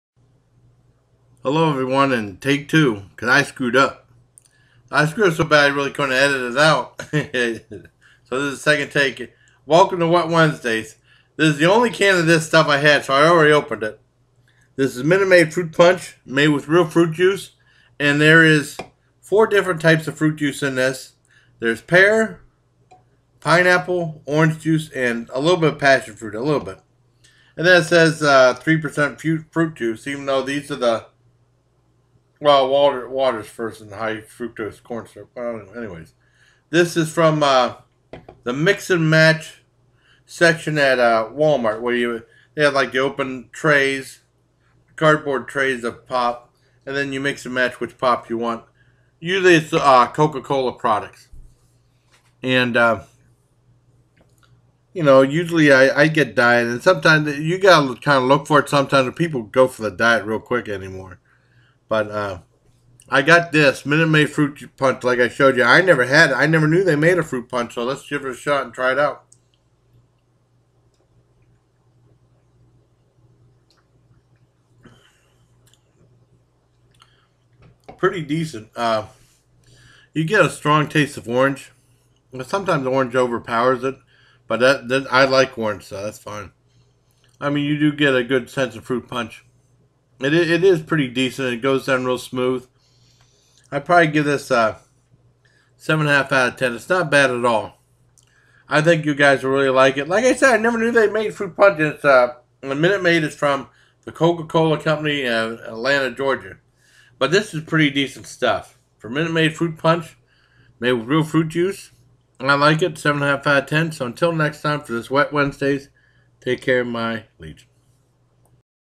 Minute maid fruit punch
Welcome to Wet Wednesday. Never knew minute maid made a fruit punch.